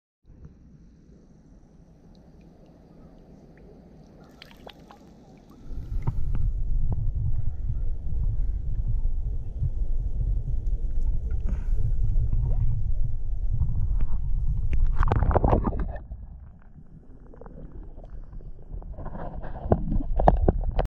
Big Ass Snail Okinawa
Wasn't sure if it was a cone snail or not. Any ideas?